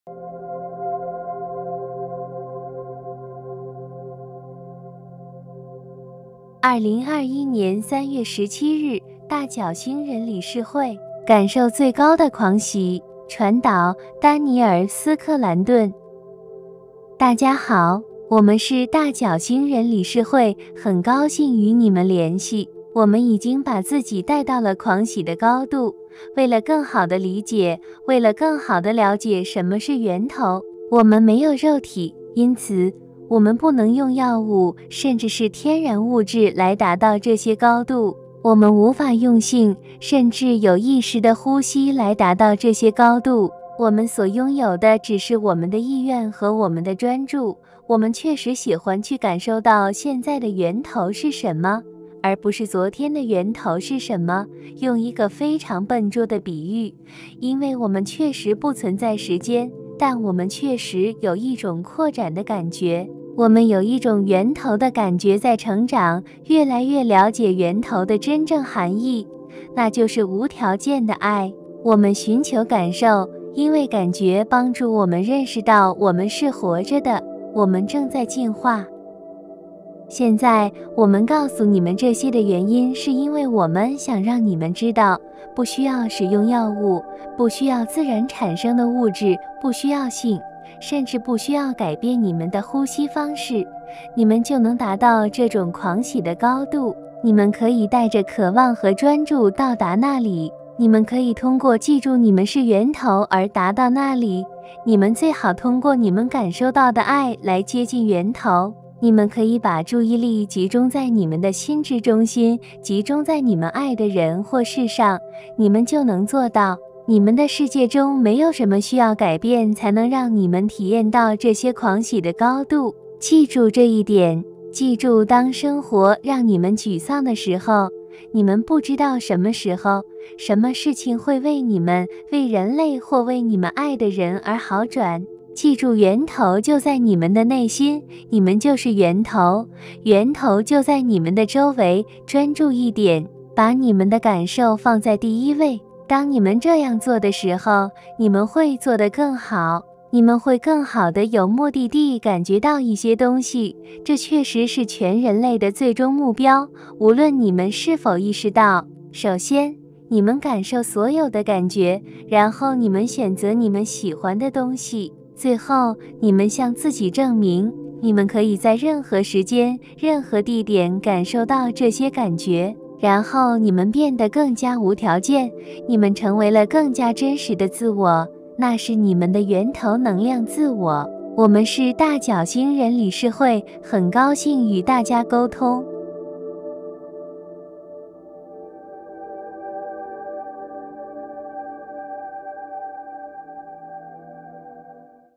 【大角星人理事會】《感受最高的狂喜》你們可以把注意力集中在你們的心之中心，集中在你們愛的人或事上，你們就能做到。你們的世界中沒有什麽需要改變才能讓你們體驗到這些狂喜的高度
2021年3月17日【大角星人理事會】

《感受最高的狂喜》

我們已經把自己帶到了狂喜的高度，爲了更好地理解，爲了更好地了解什麽是源頭。我們沒有肉體，因此，我們不能用藥物，甚至是天然物質來達到這些高度。我們無法用性，甚至有意識的呼吸來達到這些高度。

我們所擁有的只是我們的意願和我們的專注，我們確實喜歡去感受到現在的源頭是什麽，而不是昨天的源頭是什麽，用一個非常笨拙的比喻，因爲我們確實不存在時間，但我們確實有一種擴展的感覺。我們有一種源頭的感覺在成長，越來越了解源頭的真正含義，那就是無條件的愛。我們尋求感受，因爲感覺幫助我們認識到我們是活著的，我們正在進化。

現在，我們告訴你們這些的原因是因爲我們想讓你們知道，不需要使用藥物，不需要自然産生的物質，不需要性，甚至不需要改變你們的呼吸方式，你們就能達到這種狂喜的高度。你們可以帶著渴望和專注到達那裏；你們可以通過記住你們是源頭而達到那裏，你們最好通過你們感受到的愛來接近源頭。

你們可以把注意力集中在你們的心之中心，集中在你們愛的人或事上，你們就能做到。你們的世界中沒有什麽需要改變才能讓你們體驗到這些狂喜的高度。記住這一點；記住當生活讓你們沮喪的時候，你們不知道什麽時候、什麼事情會爲你們、爲人類或爲你們愛的人而好轉。記住源頭就在你們的內心，你們就是源頭，源頭就在你們的周圍，專注一點。

把你們的感受放在第一位。當你們這樣做的時候，你們會做得更好。你們會更好地有目的地感覺到一些東西，這確實是全人類的最終目標，無論你們是否意識到。首先，你們感受所有的感覺，然後你們選擇你們喜歡的東西。最後，你們向自己證明，你們可以在任何時間、任何地點感受到這些感覺。然後你們變得更加無條件，你們成為了更加真實的自我，那是你們的源頭能量自我。

原文鏈接：/feel-the-highest-heights-of-ecstasy-without-drugs-%e2%88%9ethe-9d-arcturian-council/